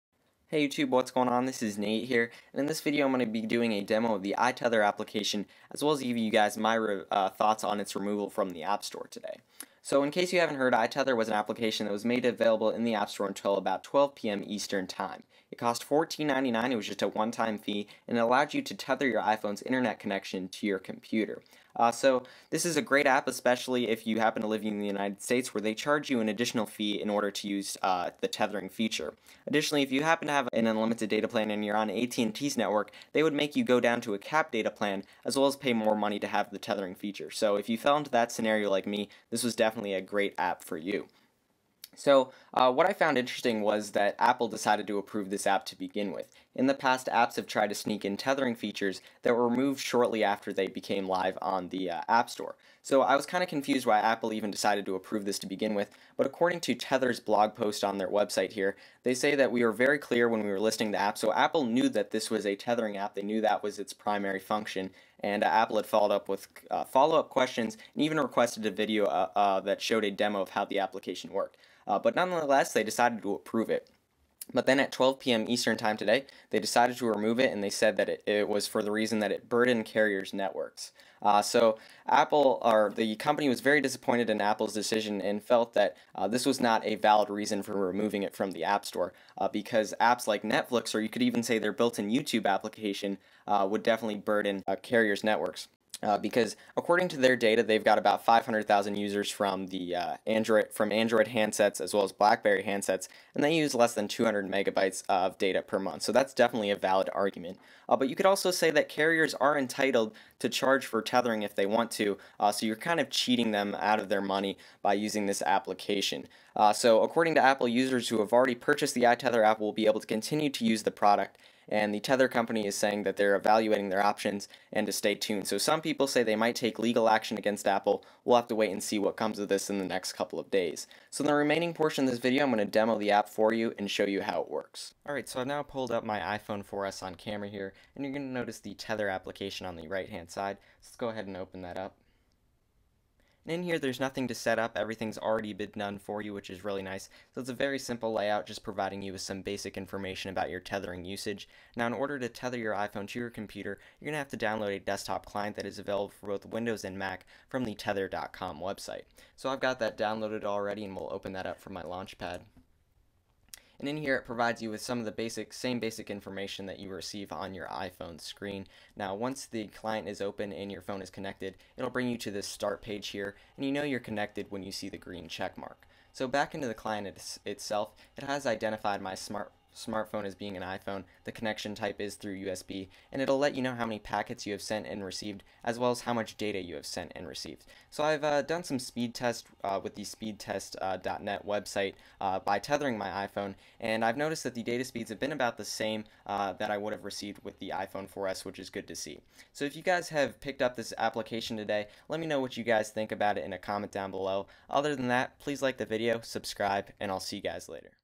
iTether Application Demo and Thoughts
The iTether application was made available in the app store today until 12 PM Eastern Time. For a price of 14.99 the app allows you to tether your iPhone to your computer without paying carrier fees. Apple removed the iTether app for being a burden to carries. It doesn't look like iTether will be made available to download again.